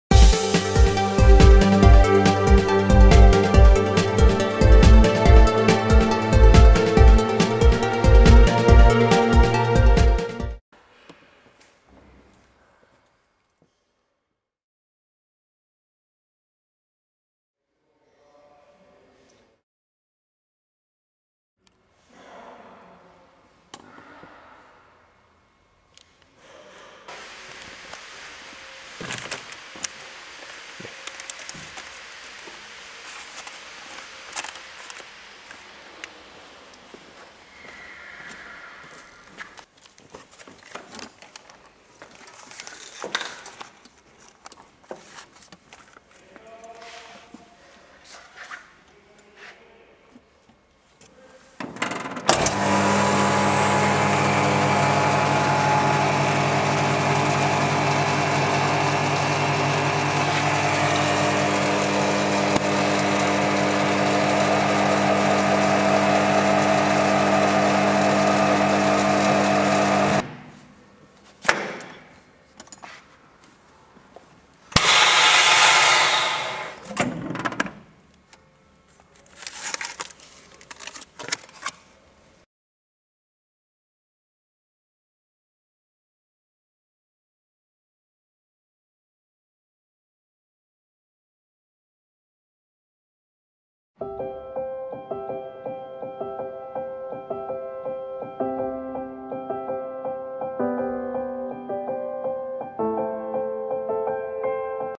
Ferplast sottovuoto DZ 400 2T
Ferplast snc Corso Asti 49/A Guarene, CN Italy

Confezionatrice sottovuoto da banco DZ 400 2T sottovuoto in acciaio inox dotata di due barre saldanti da mm. 400, pompa vuoto da 20 m3/h.

TABLE TOP VACUUM PACKAGING MACHINE DZ 400-2T Vacuum packaging machine bench with two sealing bars side.